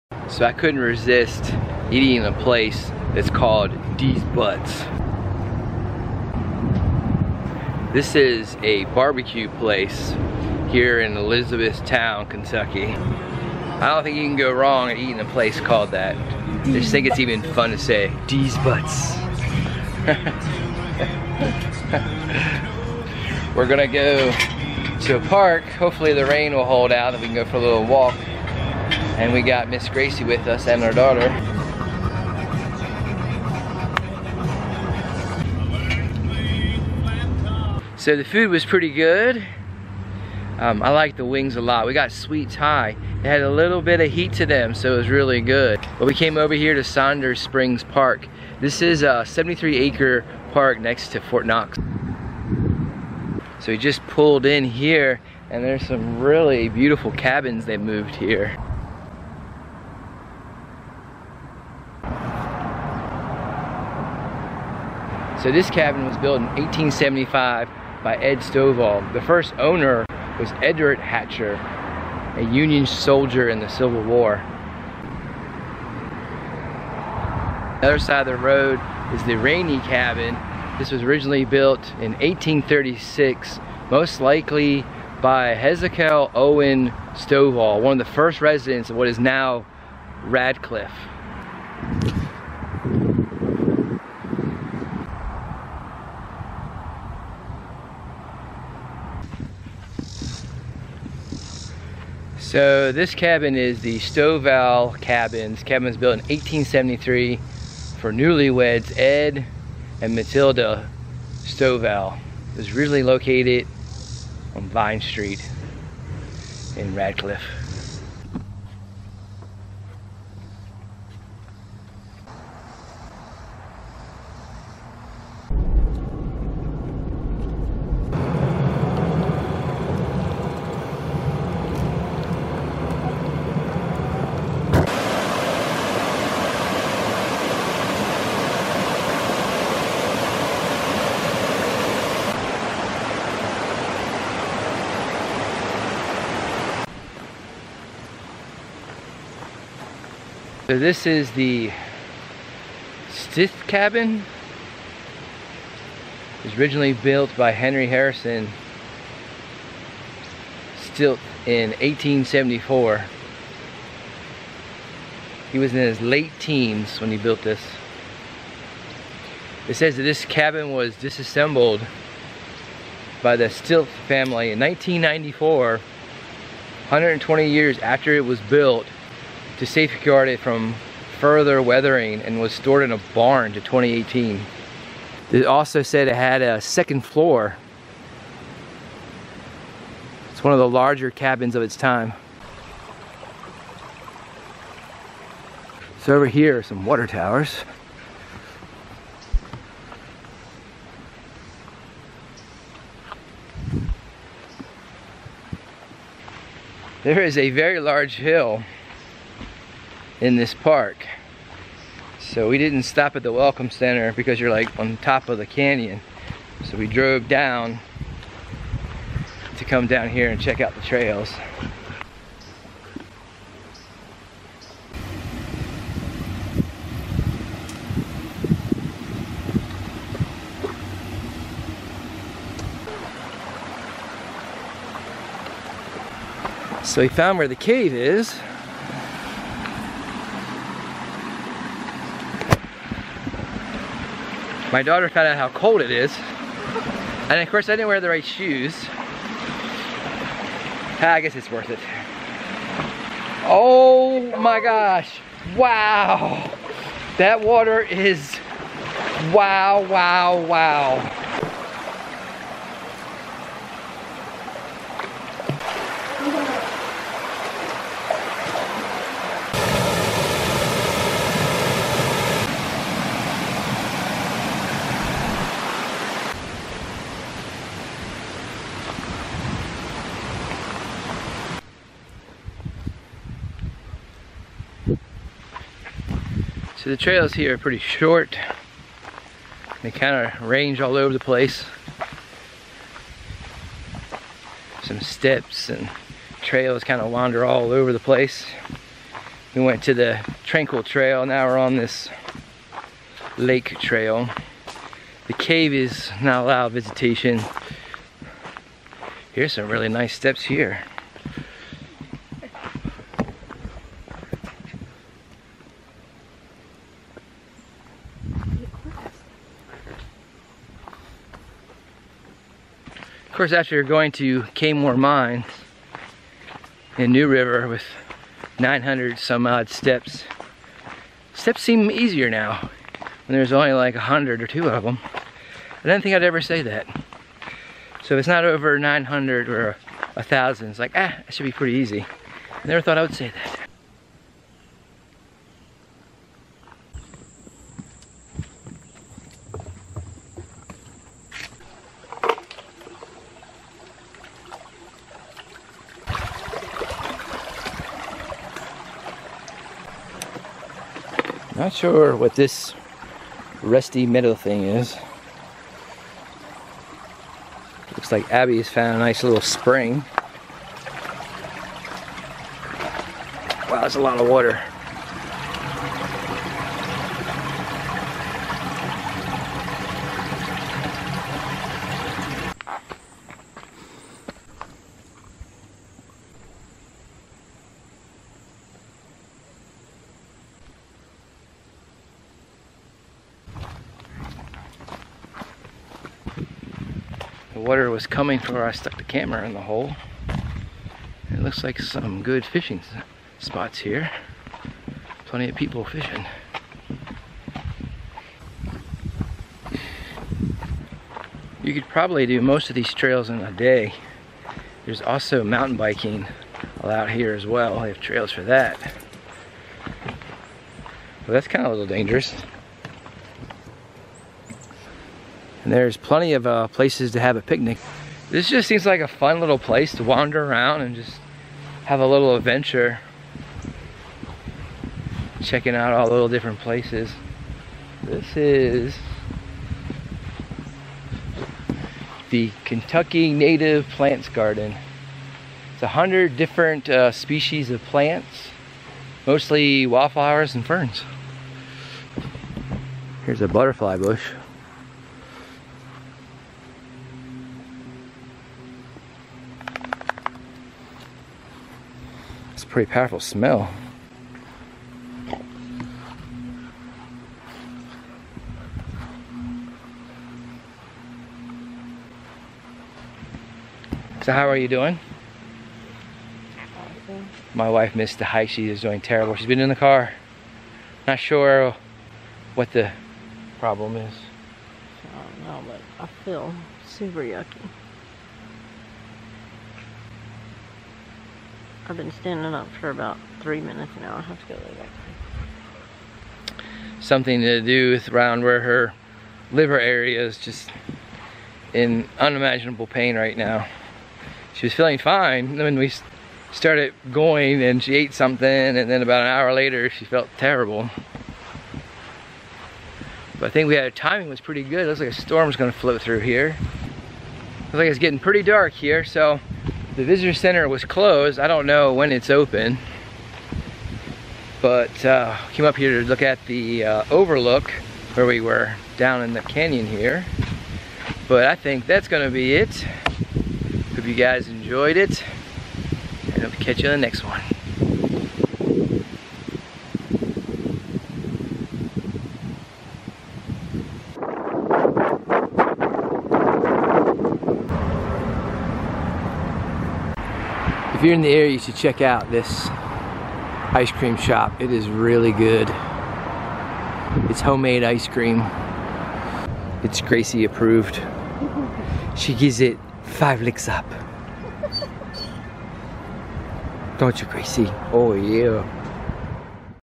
Everyone Had A Good Time Except For My Wife/Deez Butts BBQ/Saunders Springs KY
I might be a corn ball but eating out at a place called Deez Butts is just funny to me and the little park was great with the historic cabins. Sadly the cave was closed.  That could of been fun to explore. But even though it was a small park it was a satisfying place to visit. Unfortunately my wife was very sick and didn't get to enjoy the food or the park. We believe she had an infection in her pancreas.